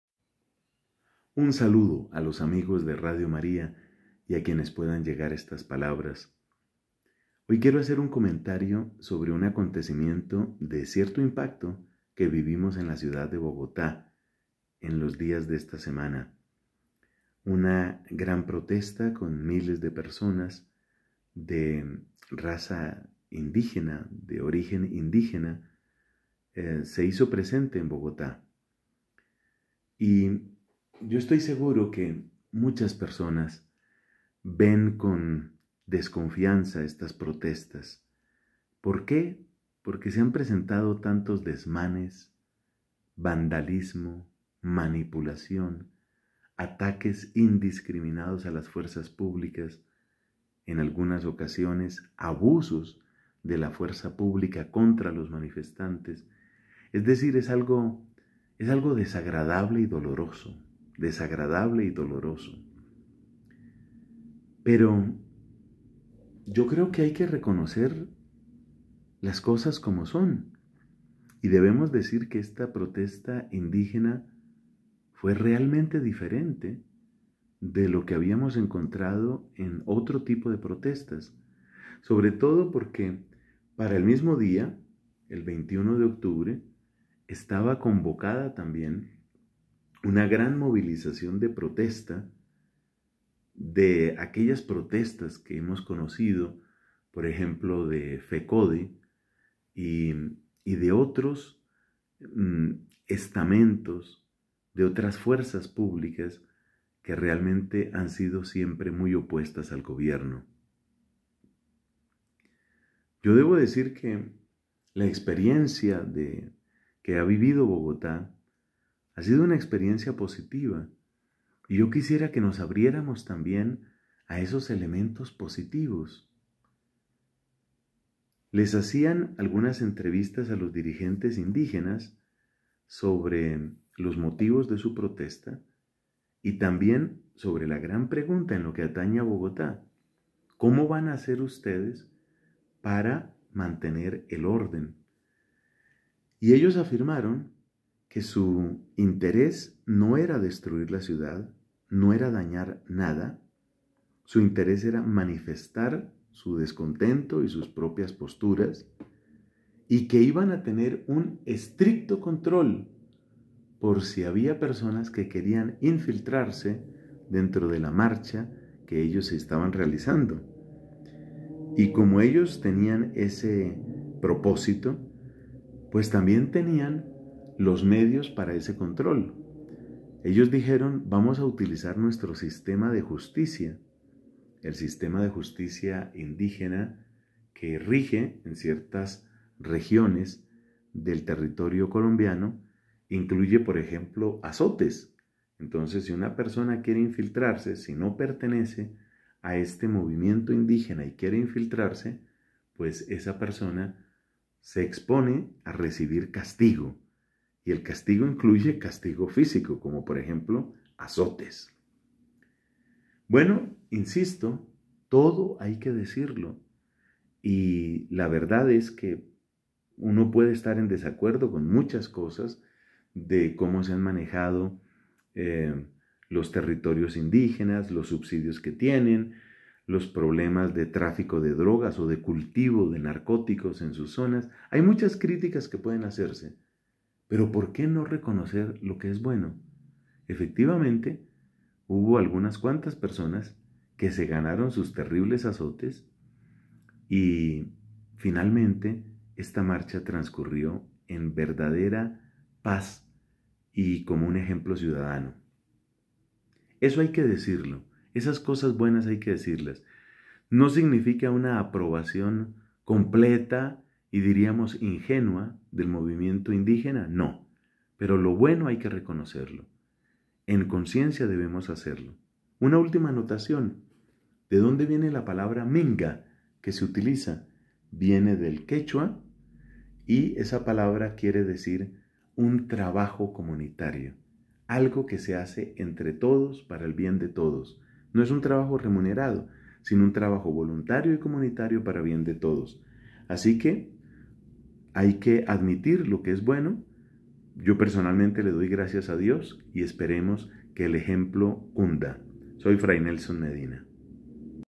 (038) Marcha indígena pacífica en Bogotá - Noticia leída desde la fe
La Minga indígena que se hizo presente en la capital de Colombia dejó un testimonio de orden y protesta pacífica. Análisis de fray Nelson Medina.